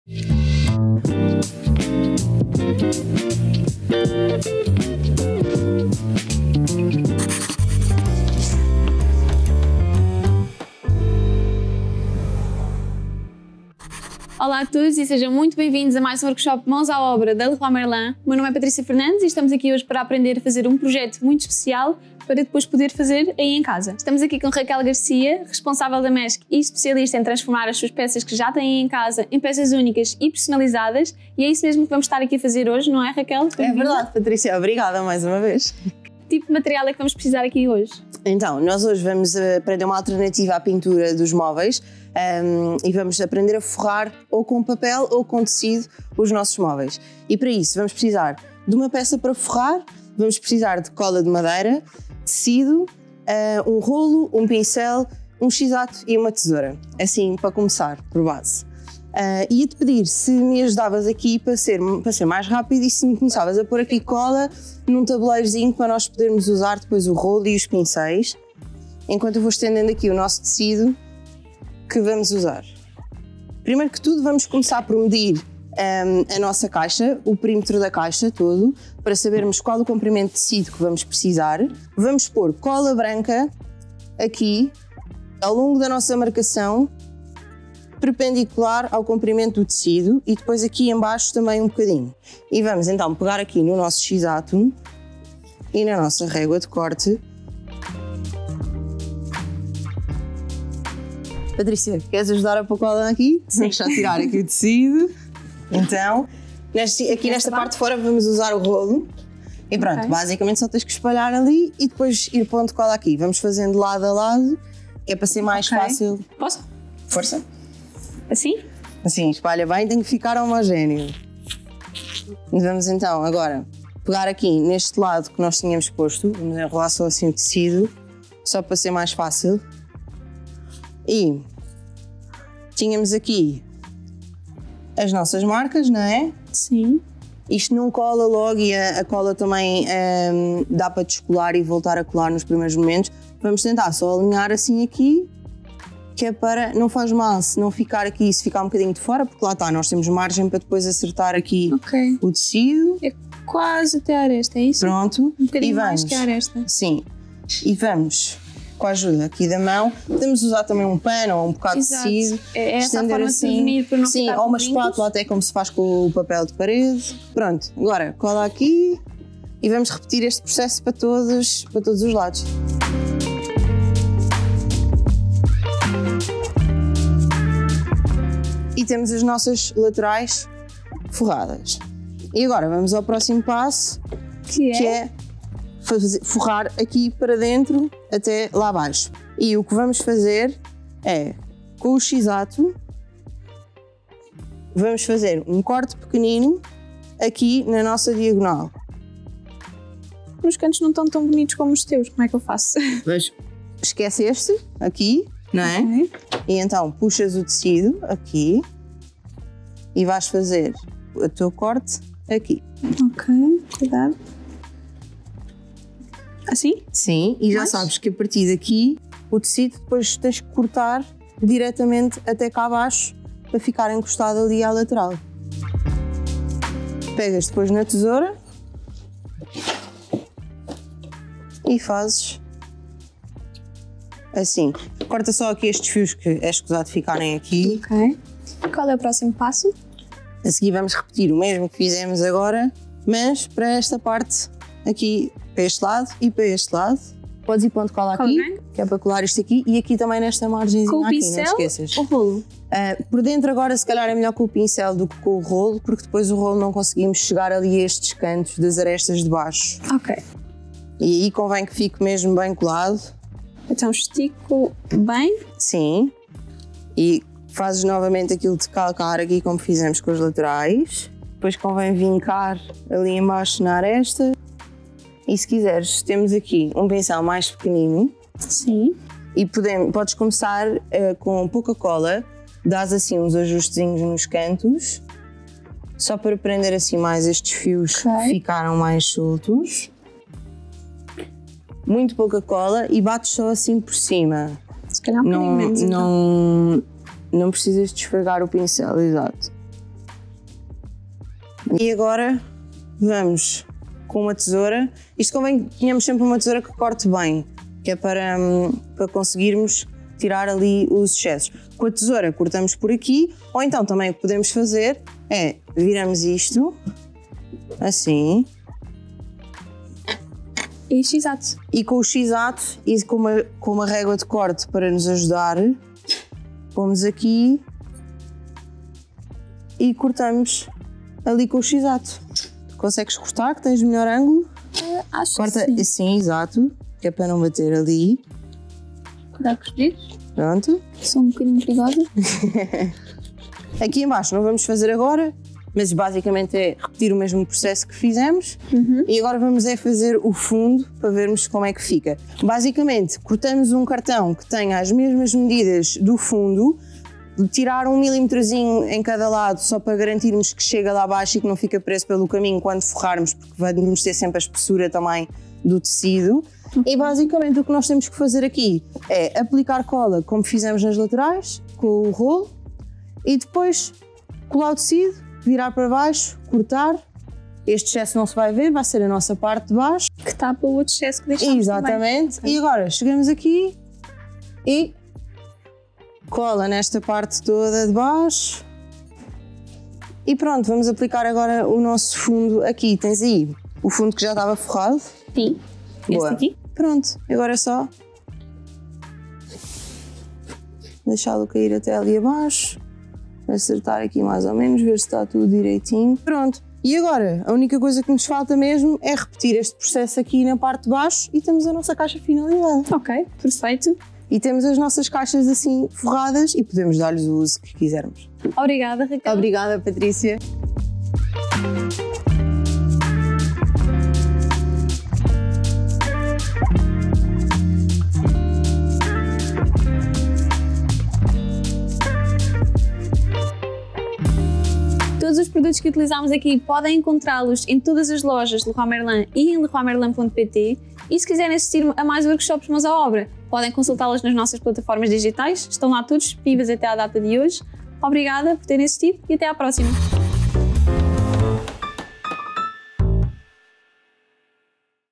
Como Forrar Móveis c/Tecido ou Papel Decorativo | Mãos à Obra LEROY MERLIN
Sem ideias para dar uma nova vida ao móveis antigos? A Raquel Garcia, da MESQUE, vai ensinar uma técnica para forrar qualquer tipo de peça decorativa com tecido, personalizando-a a gosto.

00:00 Apresentação do projeto e materiais necessários
01:14 Passo 1: Medir o móvel e marcar o tecido
01:37 Passo 2: Cortar o tecido
01:56 Passo 3: Aplicar cola e de seguida o tecido
03:15 Passo 4: Ajustar os cortes e aplicar a camada interior
04:55 Passo 5: Acertar os cantos
06:03 Passo 6: Fazer um fundo de cartão revestido